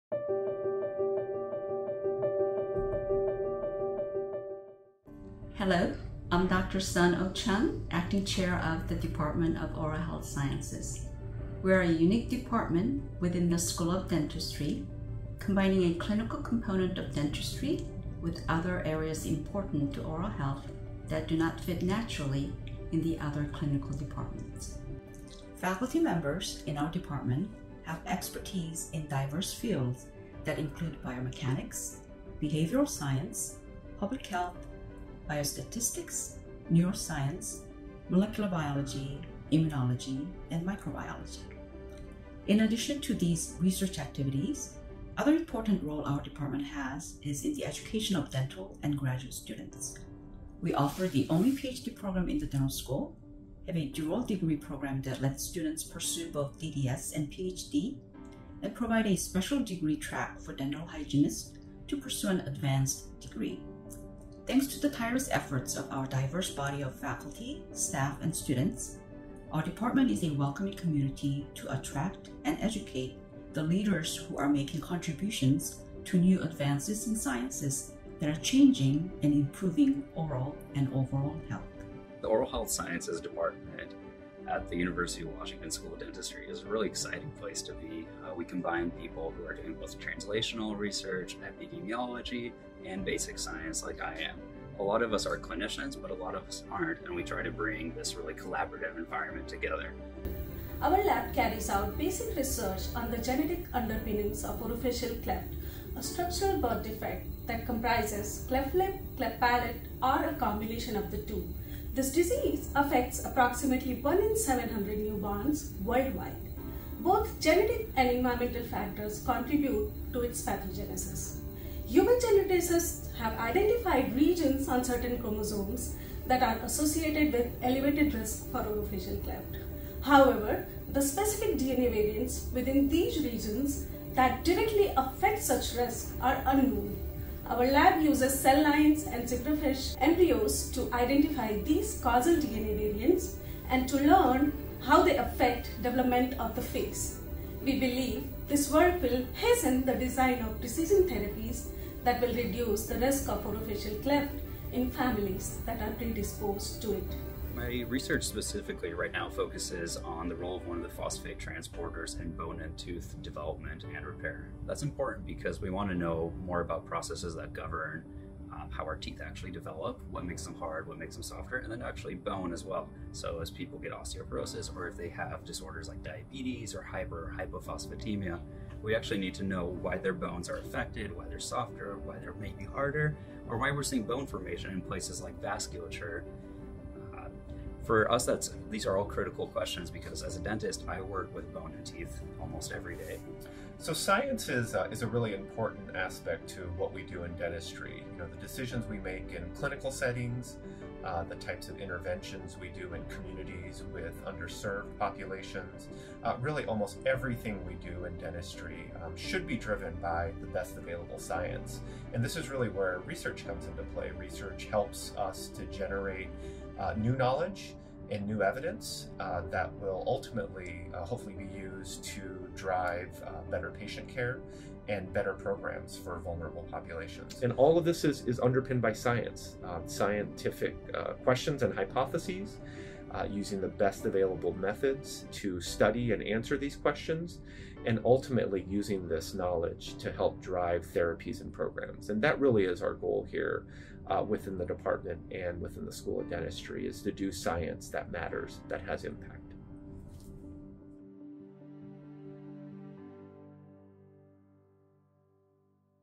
Welcome to UW Oral Health Sciences
The Department of Oral Health Sciences at the University of Washington School of Dentistry is a hub of research that helps provide the foundation for advances in dental practice and oral health.